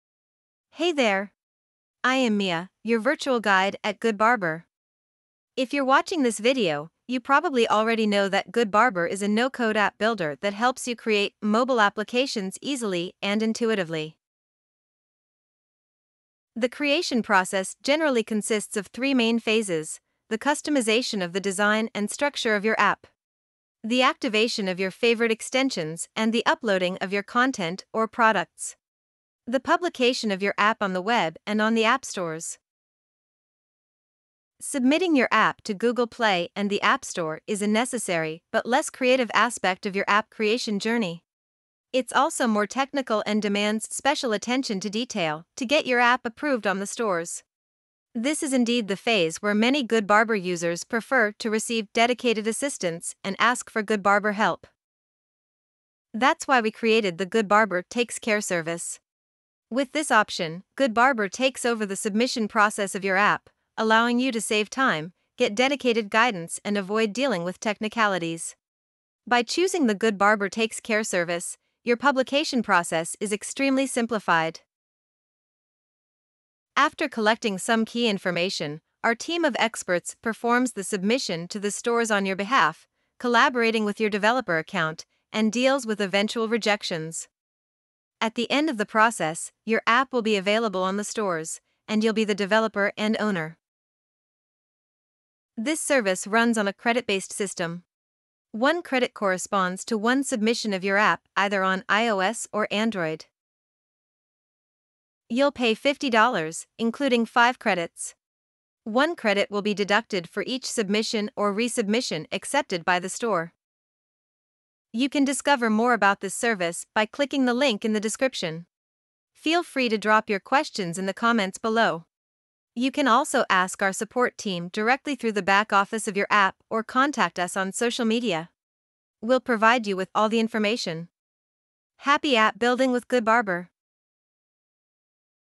🇺🇸 Publish your mobile app with "GoodBarber Takes Care"
Time to publish your app on Google Play and the App Store? We’ve got you covered! 🙌🏼

Save time and avoid dealing with technicalities ➔ 𝗚𝗼𝗼𝗱𝗕𝗮𝗿𝗯𝗲𝗿 𝘁𝗮𝗸𝗲𝘀 𝗼𝘃𝗲𝗿 𝘁𝗵𝗲 𝘀𝘂𝗯𝗺𝗶𝘀𝘀𝗶𝗼𝗻 𝗽𝗿𝗼𝗰𝗲𝘀𝘀 𝗼𝗳 𝘆𝗼𝘂𝗿 𝗮𝗽𝗽 with GBTC (GoodBarber Takes Care service).